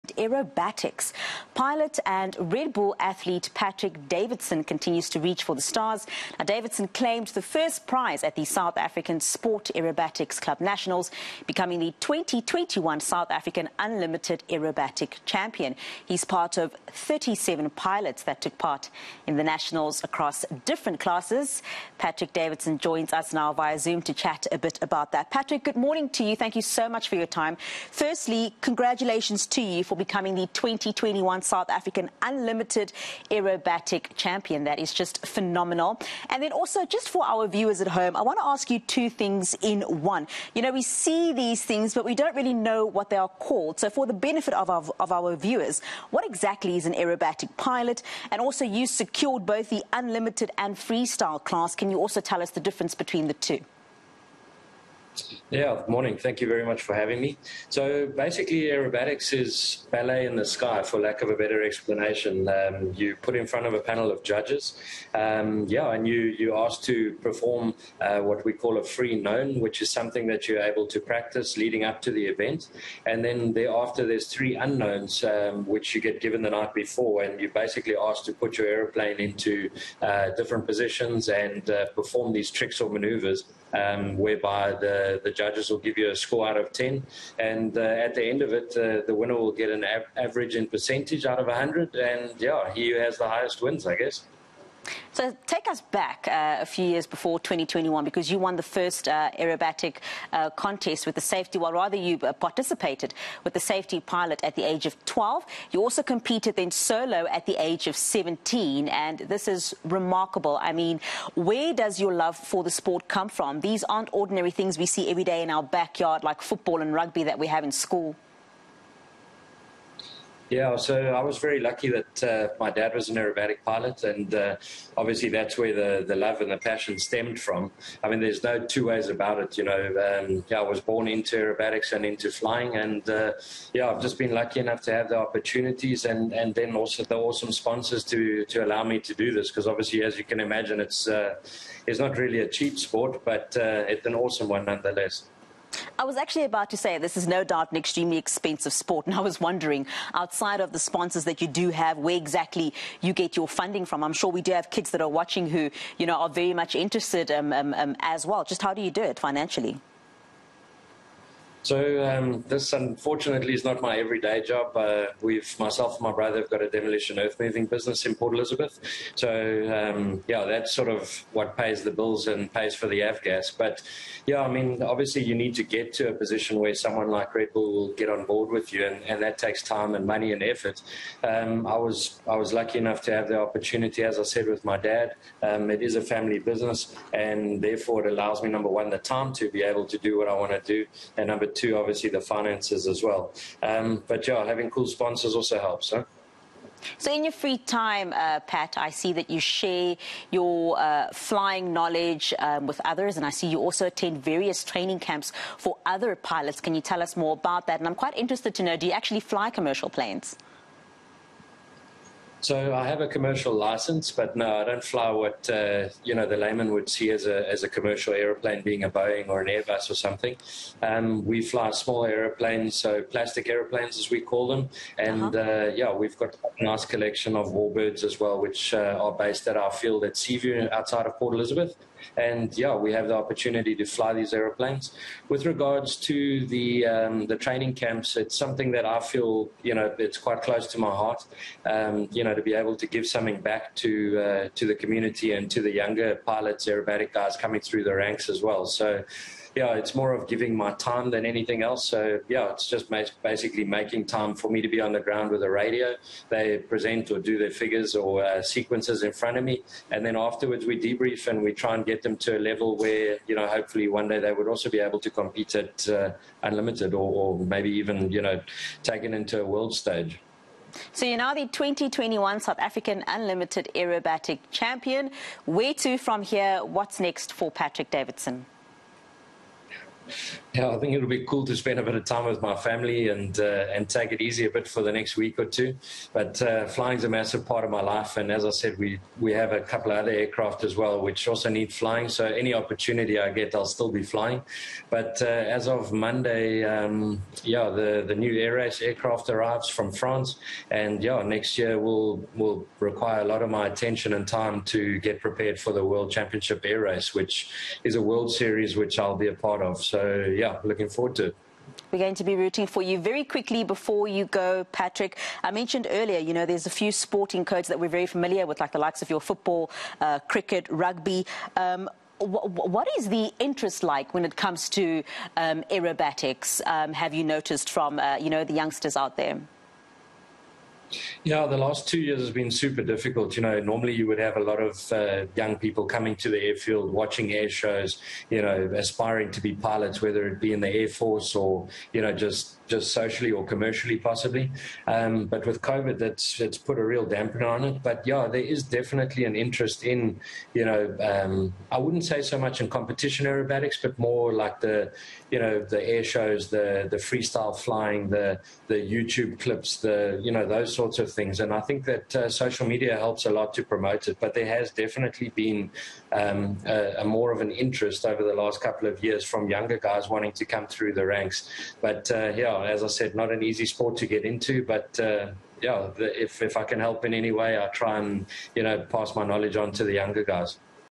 Aerobatics SA | Davidson clinches 2021 championship
Pilot and Red Bull Athlete Patrick Davidson continues to reach for the stars. Davidson claimed first prize at the SA Sport Aerobatics Club Nationals, becoming the 2021 South African Unlimited Aerobatic Champion. He speaks to eNCA’s Nicole Kente. Courtesy